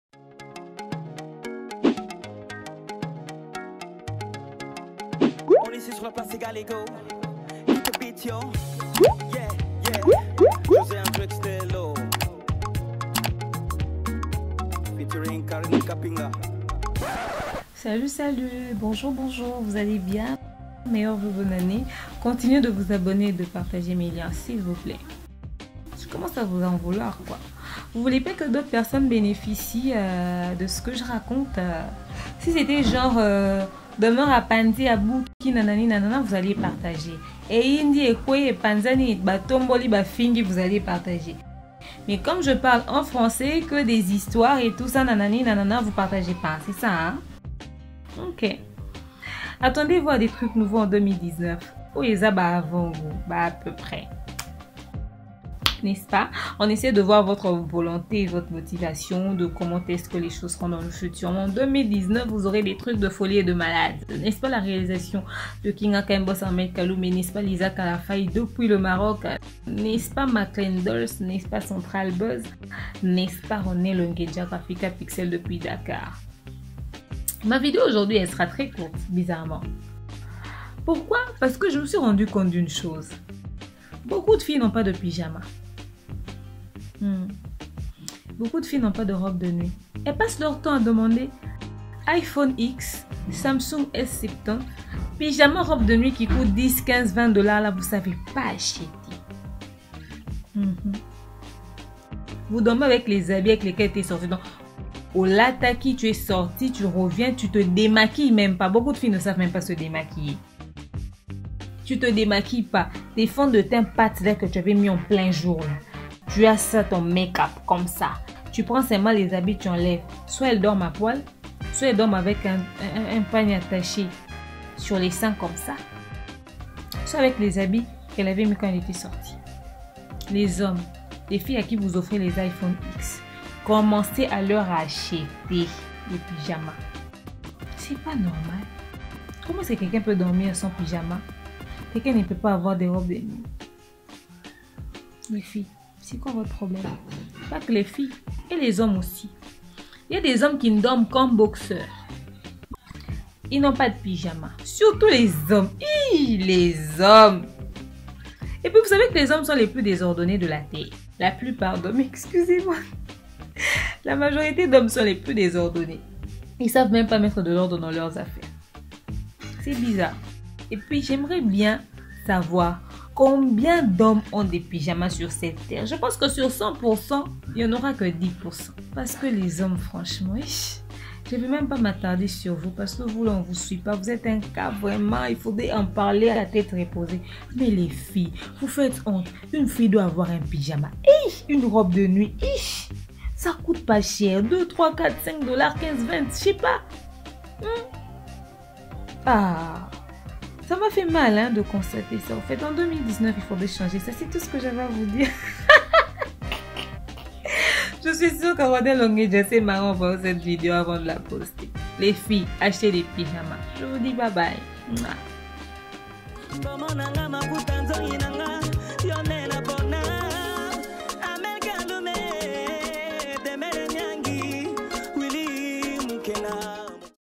ACTE6: LES FILLES ANTI- PYJAMA et ANTI-ROBE DE NUIT
A vous qui aimez toujours dormir comme dans un camp de militaire aux foyers
AVEC DEMEURE NIANGI MUKUSE YA TALO#

Education, Social , société, gag, rire, humour, Etc.
Production: LONGENDJA-GRAPHICAPIXEL
Coproduction: Armel KALUMEI
Présentatrice : Demeure NIANGI
DESIGN MOTION: Rooney Longendja
Dakar-kinshasa 2018